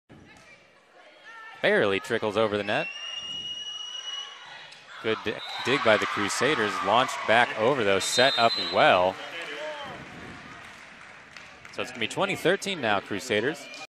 Faith Lutheran #8 Gianni Giuliano with the outside kill
Faith Lutheran senior OH #8 Gianni Giuliano with the big outside kill in game 1 vs. Dayton High School.